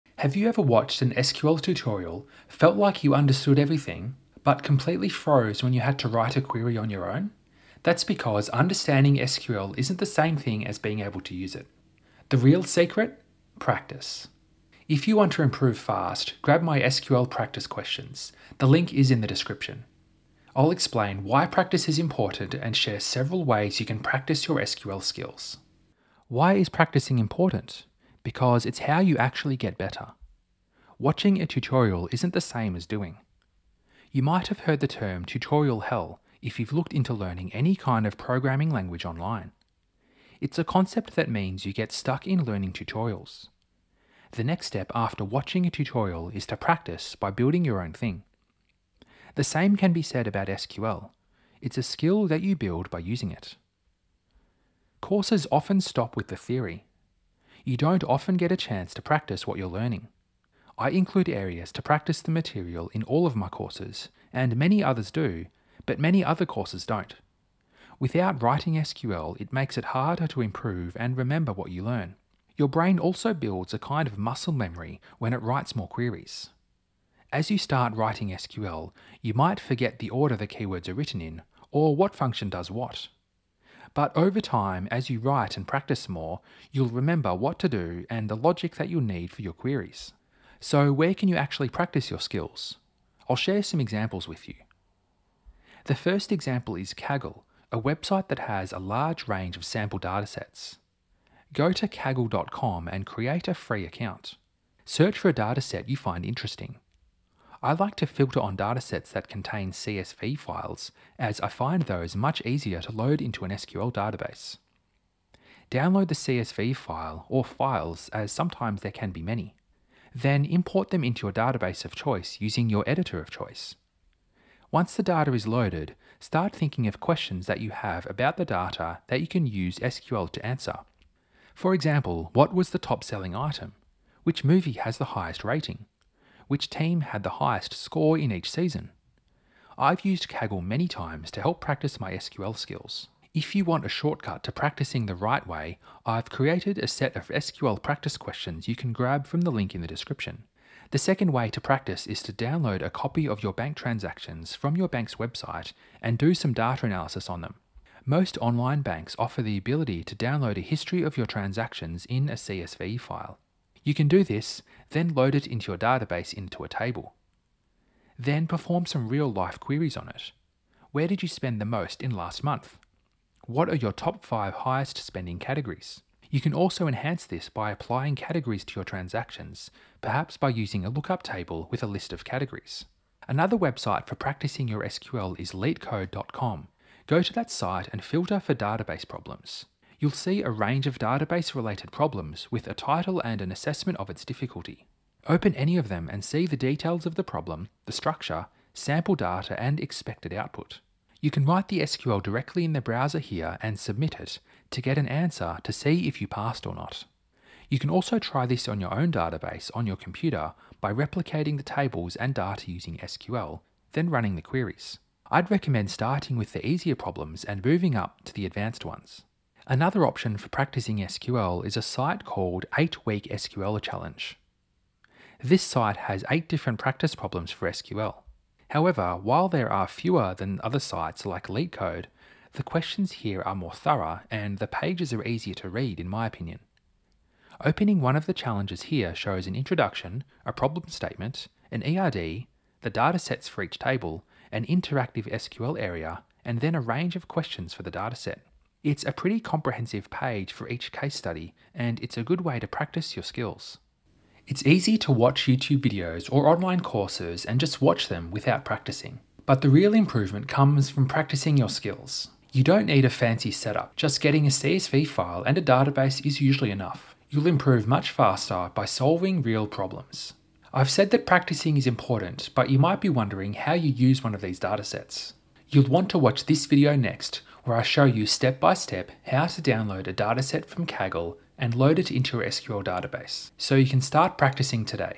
The Secret to Writing Better SQL Queries
In this video, I explain what I think is the best way to improve your SQL skills and how to do it.

⏱ TIMESTAMPS:
00:00 - Benefits
01:34 - Site 1
02:30 - Site 2
03:03 - Site 3
03:38 - Site 4